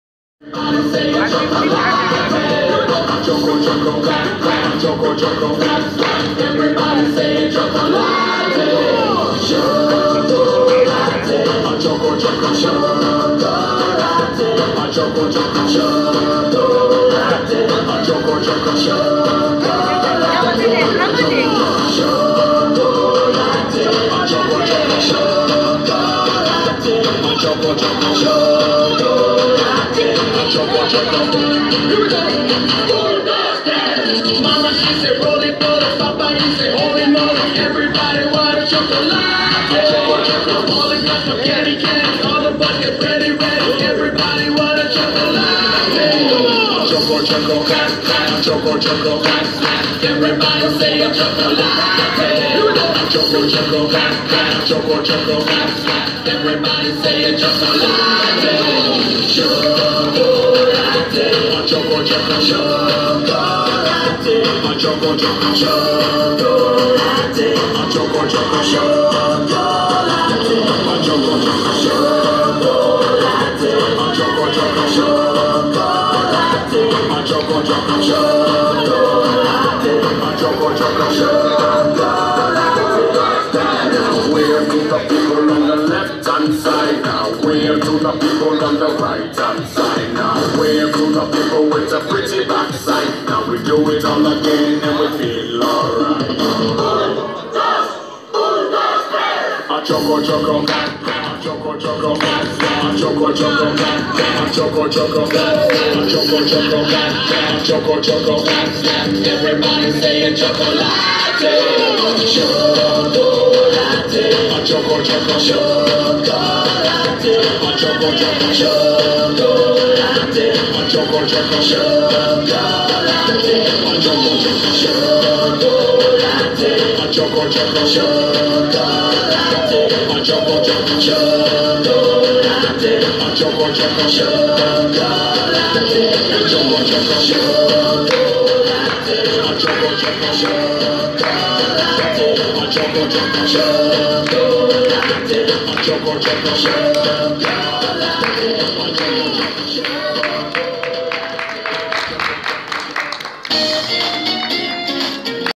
Limak Lara Deluxe
Kinder Disco, Hotel Limak Lara, Schaum Party, Musik, Tanz, Spaß, Party, Türkei.